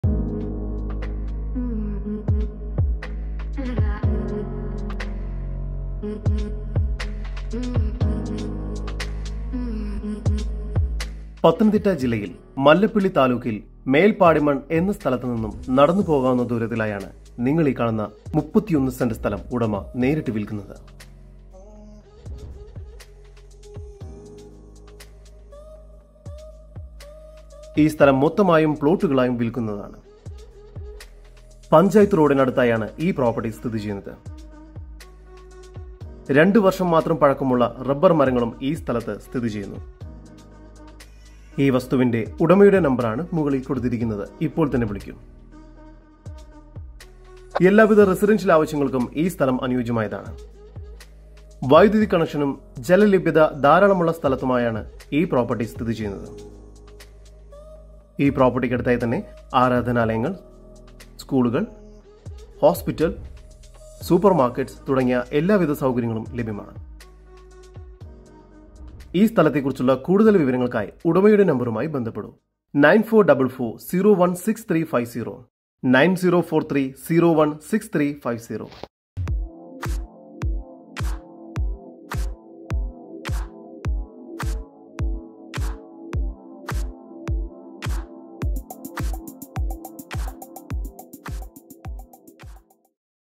Plot Sale In Pathanamthitta Mallappally | BY Owner
Plot Sale In Pathanamthitta
Mallappally
31 Cent
Plots Available
Panchayath Road Access
Residential Plot
Water & Electricity
Asking Price
Owner Name   : Mr. Thomas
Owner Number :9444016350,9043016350